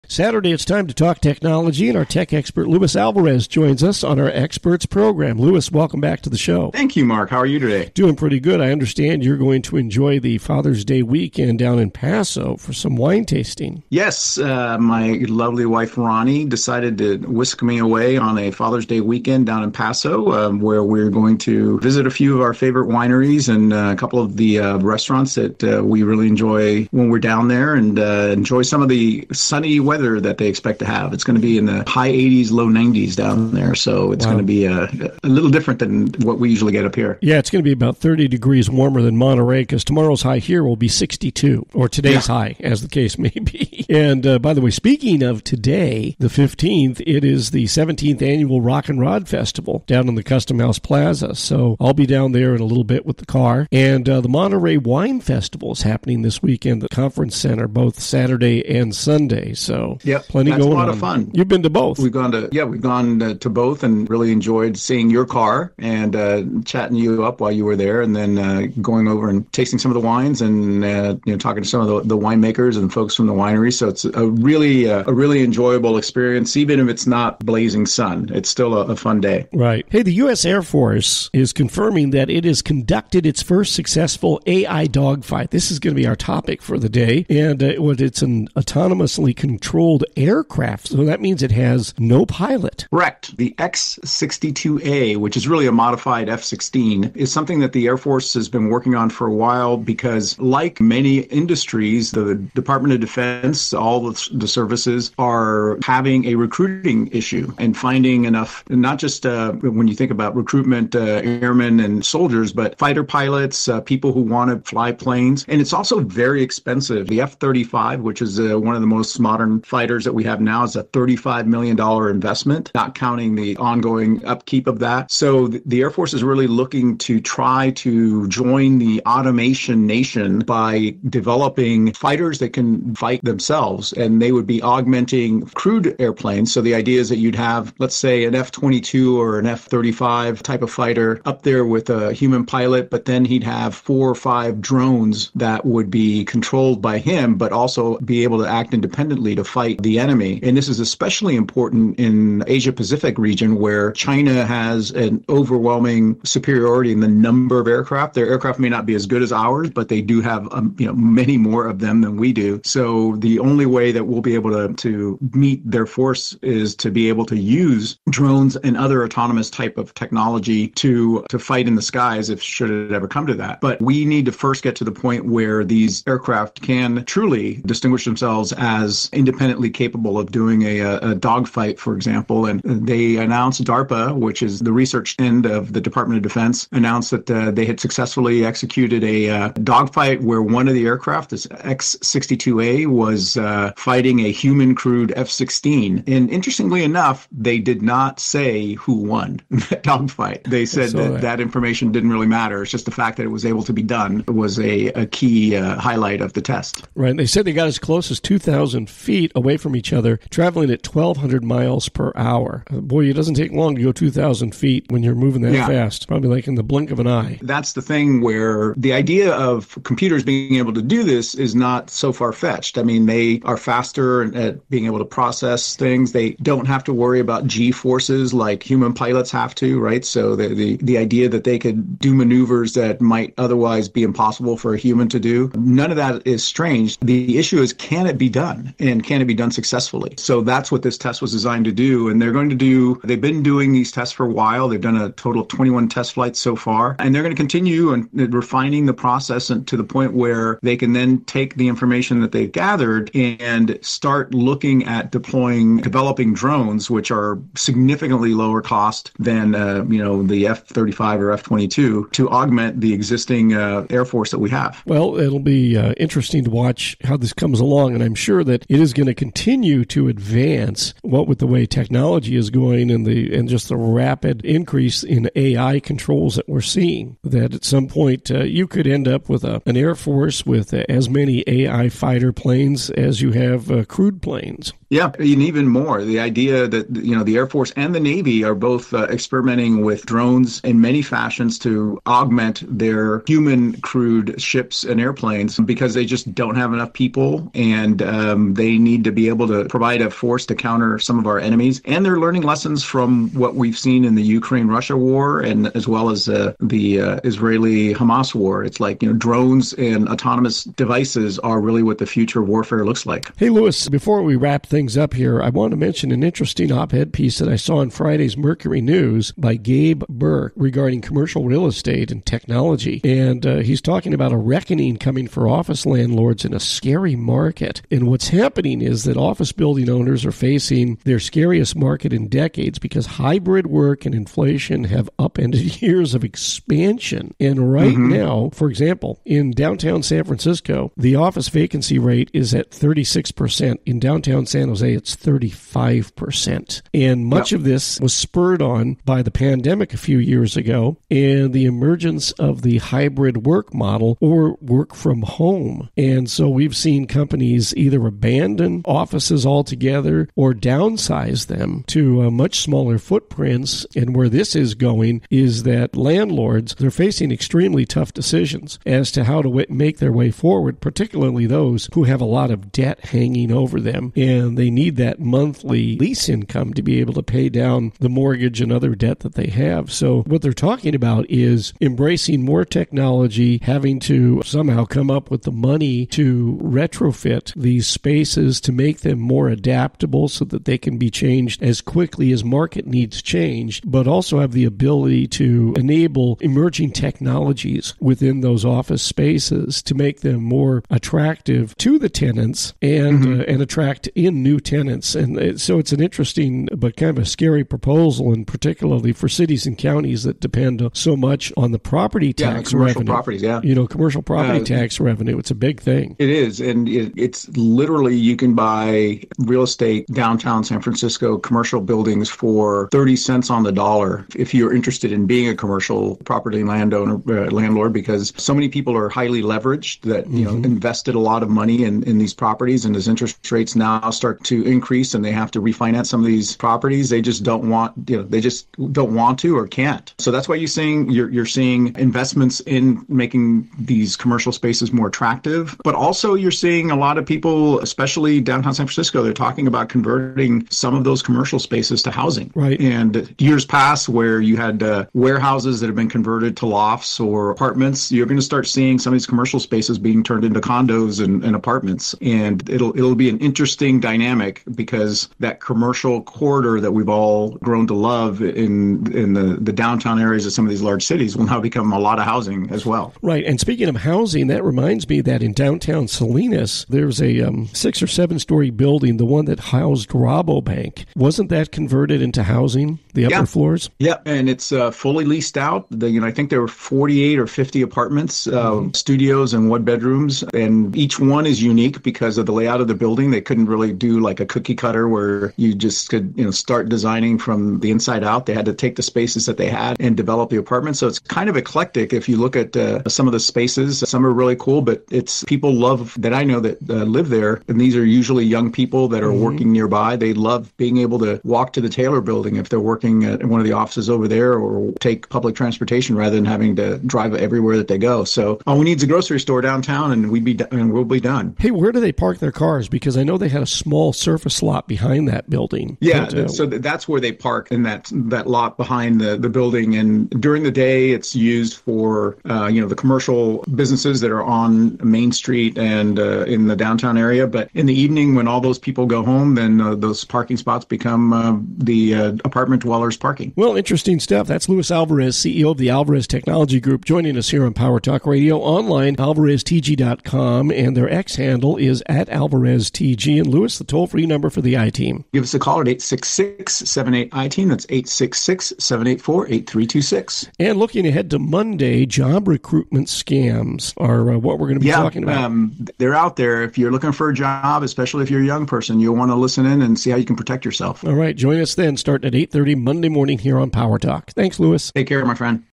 United States Air Force Concludes First Ever AI Dog Fight
US Air Force Conducts First Ever AI Dogfight: A Milestone in Military Technology

The US Air Force has made history by conducting the first-ever dogfight between an artificial intelligence and a human pilot. In this groundbreaking event, a modified F-16, known as the X-62A, was flown autonomously by AI and engaged in aerial combat with a human-piloted jet. This development was part of a series of tests at Edwards Air Force Base.

The AI-controlled aircraft, developed by the Defense Advanced Research Projects Agency (DARPA), demonstrated impressive capabilities by matching the speed and agility of its human counterpart. The test, which saw the jets fly at up to 1,200 mph, proved that AI could transform future air combat scenarios.

This significant achievement showcases the potential for AI to assist or even replace human pilots in critical missions. As technology advances, the implications for military strategy and tactics are profound.

Luis Alvarez from the Alvarez Technology Group shares interesting technology news with the Salinas business community twice a week.